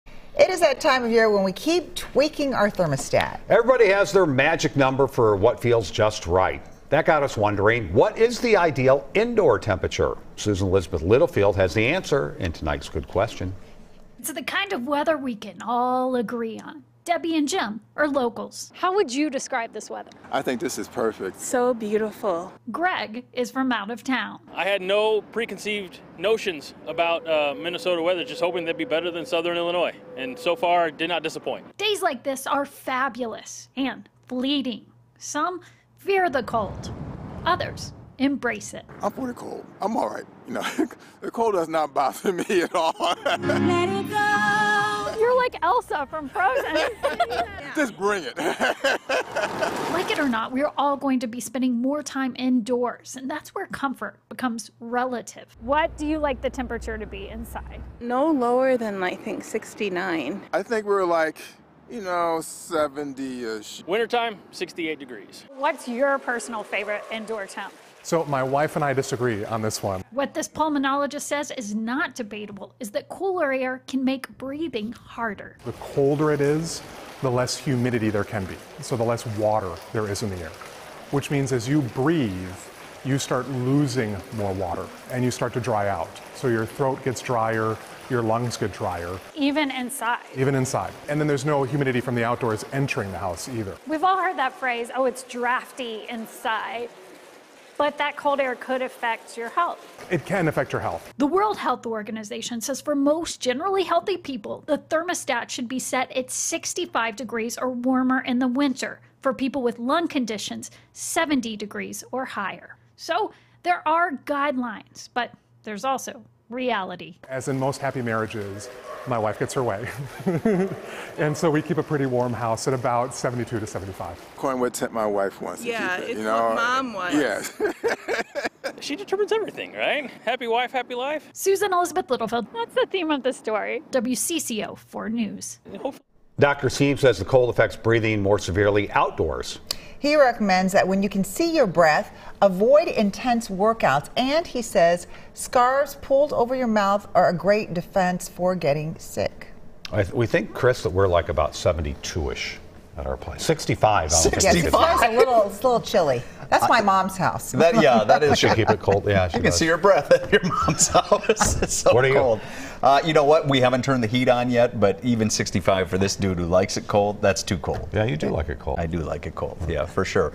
Good Question: What is the ideal indoor temperature?
It's that time of year when we keep tweaking our thermostat, so Susan-Elizabeth Littlefield spoke with pulmonologist Dr. Andrew Stiehm about the “magic number.”